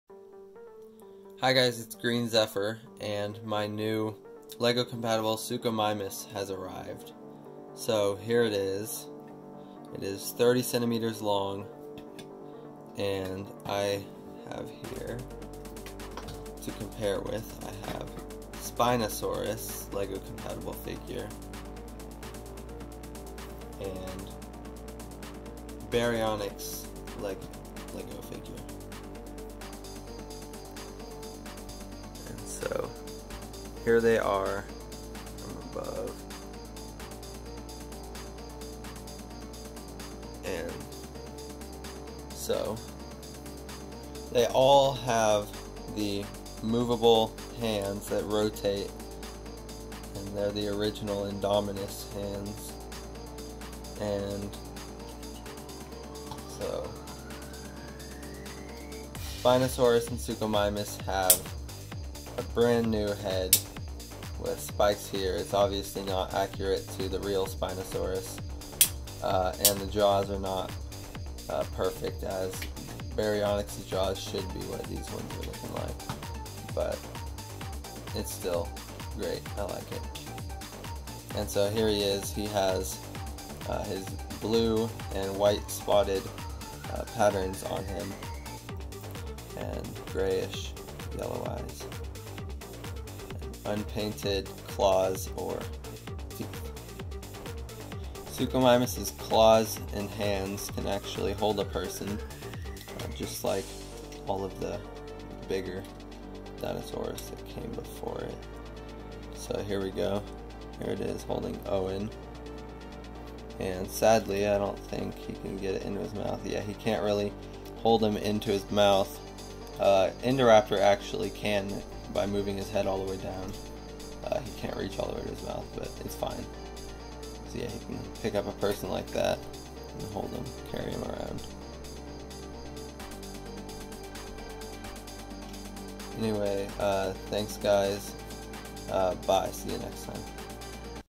New LEGO Compatible Suchomimus | LEGO Jurassic World
My LEGO compatible Suchomimus has arrived.